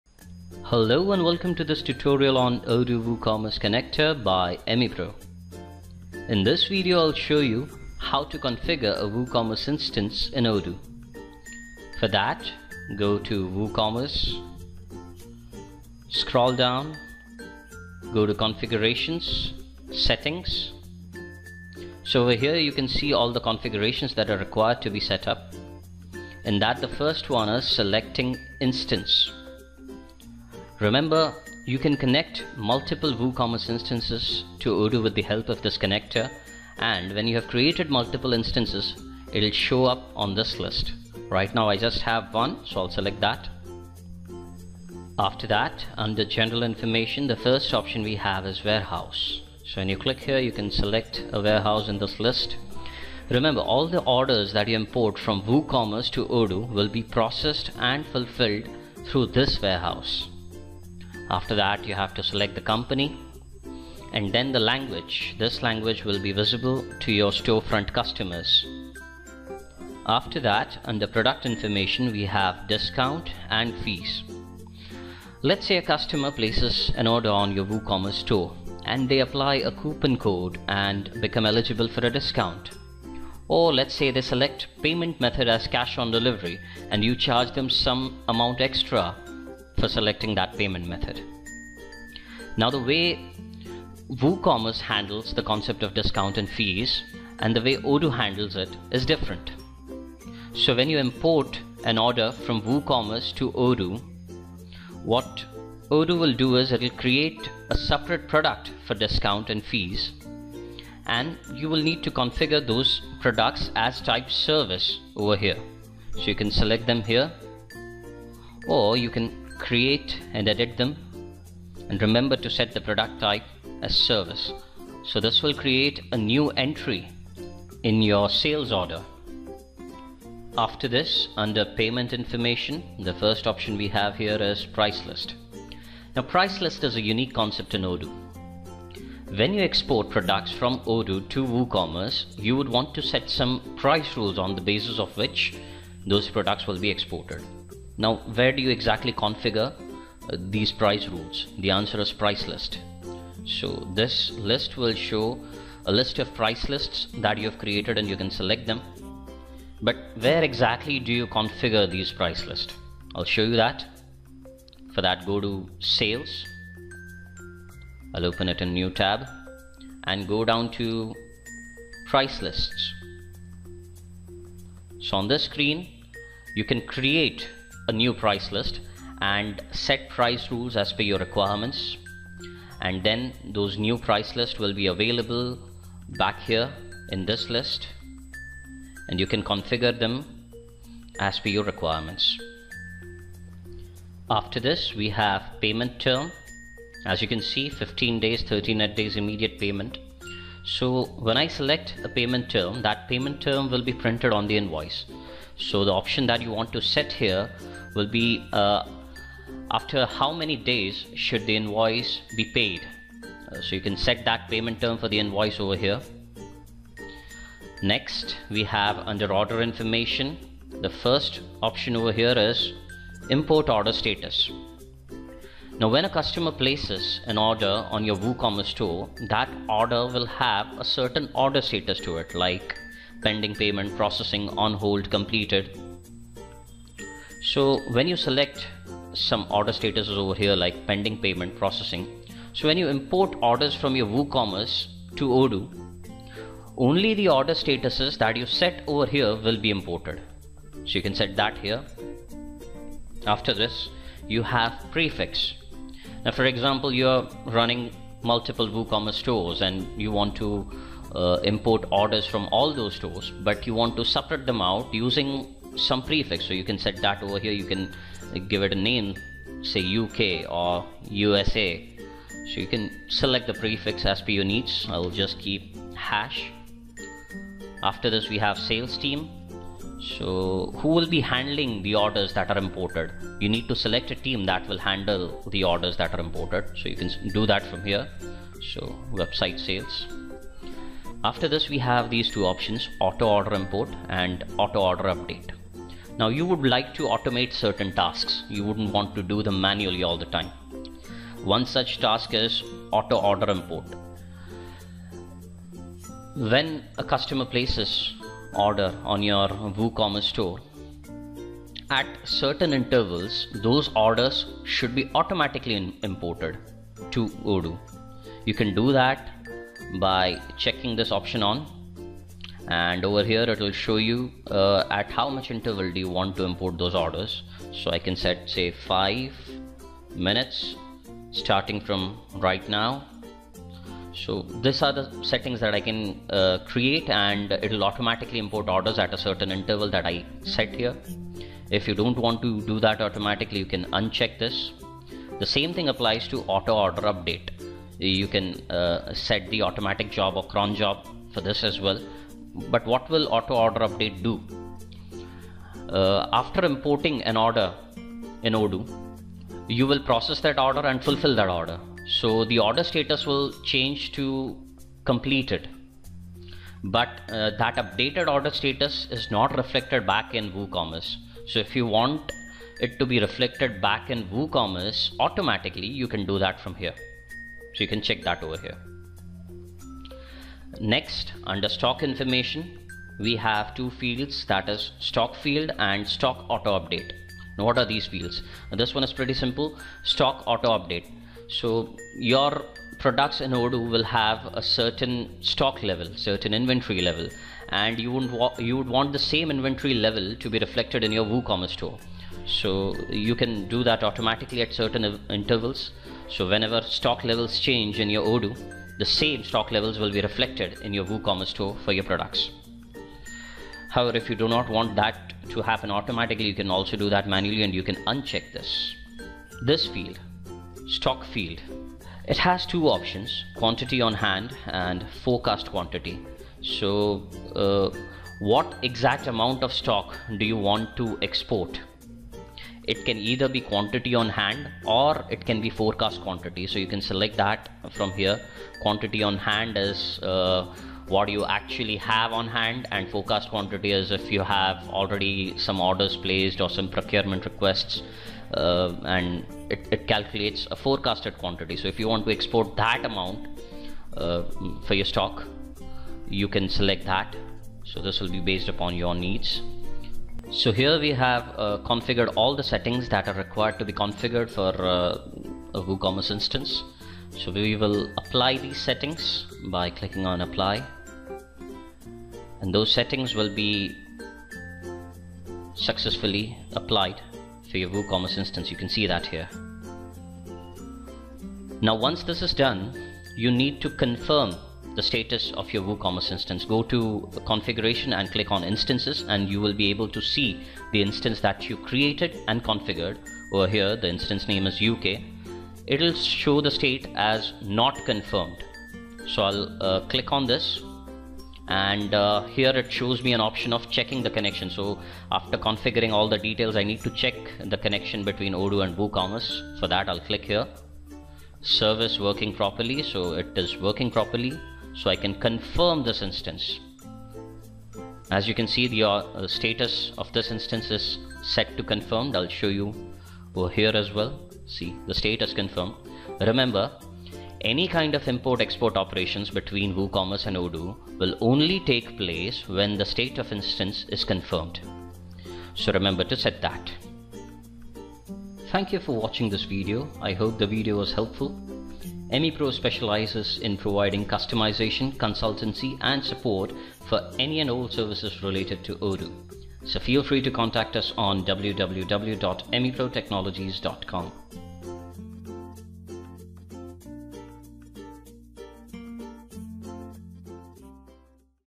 1.2 - Configuring WooCommerce Instance in Odoo | Odoo WooCommerce Connector
User guide of Odoo WooCommerce Connector:

Once you install WooCommerce Odoo Connector from Apps / WooCommerce in Odoo, you will be asked to setup your WooCommerce Instance. When your connection is checked and confirmed while creating instance, it will load default configurations automatically which can be further modified. So, it is most important configuration to use Odoo WooCommerce Connector flawlessly. Watch this video very carefully!

Emipro specializes in providing end to end solutions in Odoo.